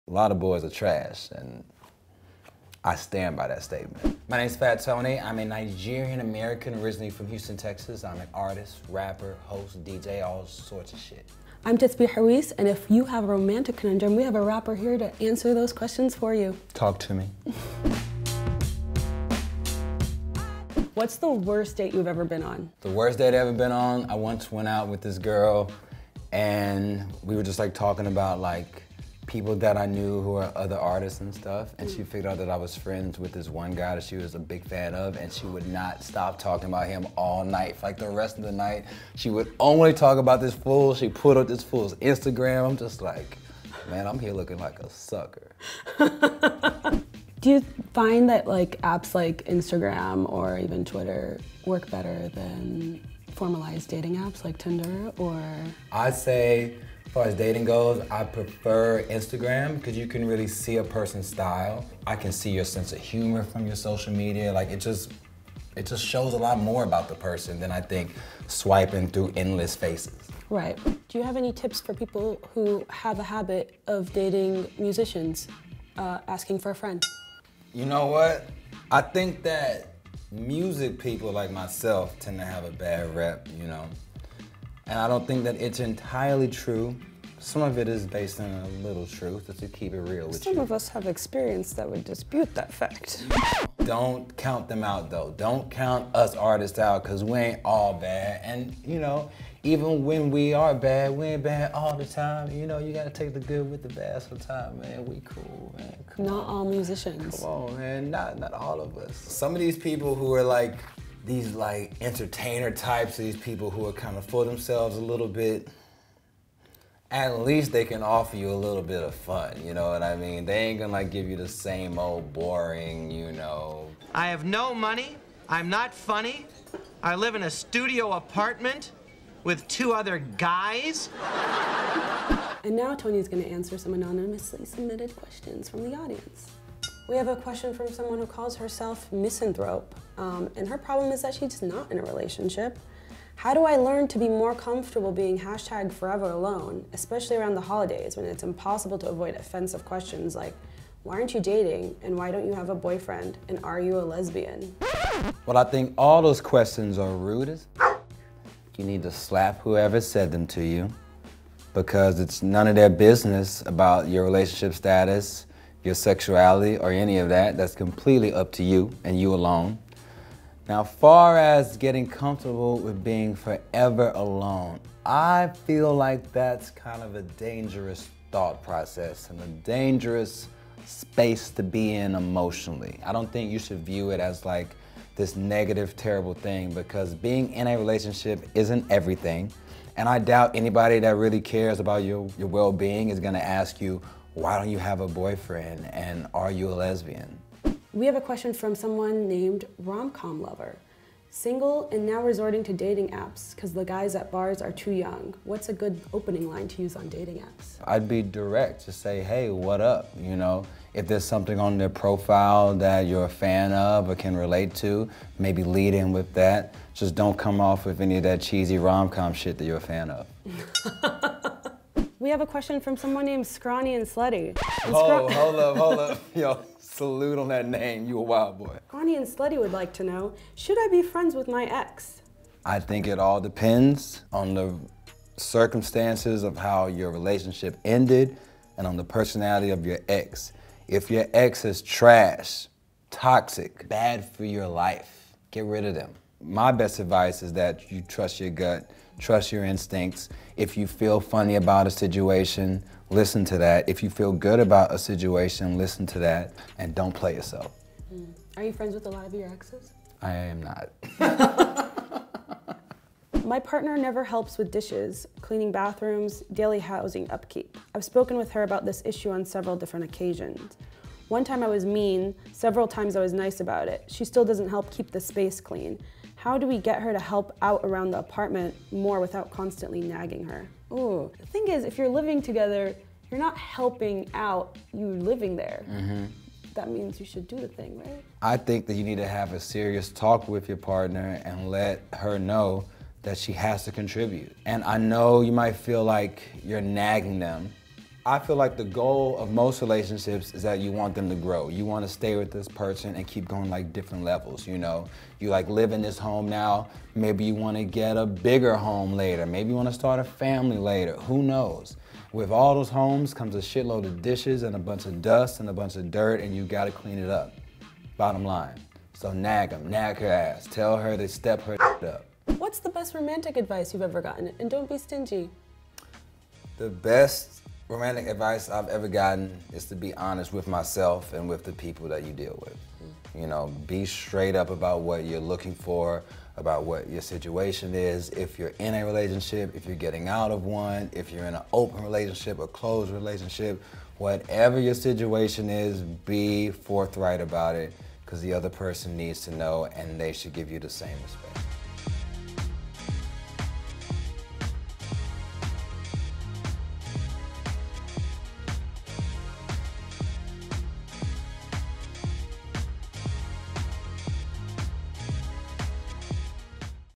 Rappers Give Dating Advice Ft. Fat Tony
"A lot of boys are trash."

MUSIC
SFX Provided By AudioBlocks

STILLS
painted heart
dimitris_k/Getty Images

EXTERNAL CREDITS
Fat Tony